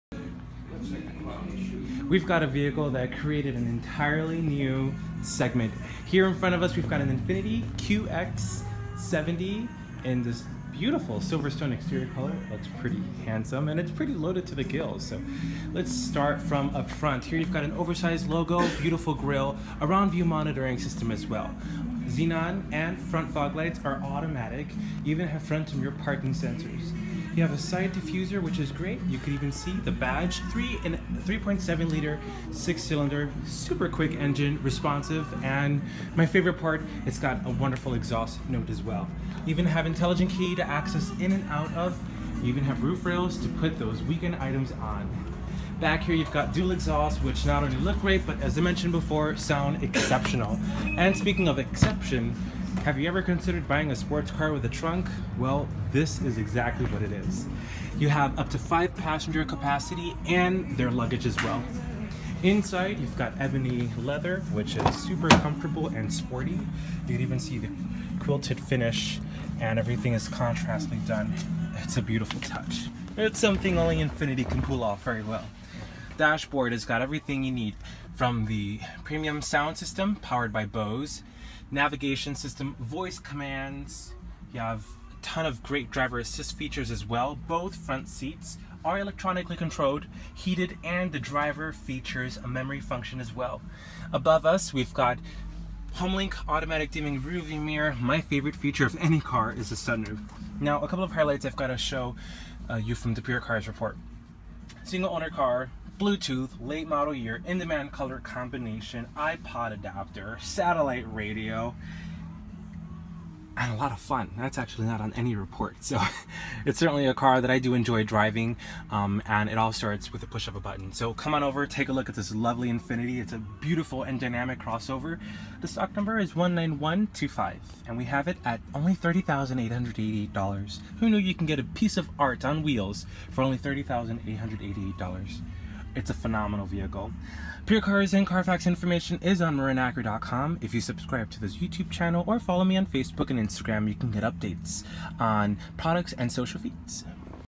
2014 Infiniti QX70 stk 19125 DM
As a fan and previous owner to an Infiniti product, I assure this will be your next car you’ll fall in love with. It offers a true sports car experience with a bit of cargo space. Rear wheel drive, powerful V6, and superb exhaust note make each ride feel like your 16 again. Single CarFax previous owner, late model year, and plenty of tech toys to keep you busy. Navigation system, Around View Monitor, sunroof, power tailgate, Bose stereo, and more. Its just a delight.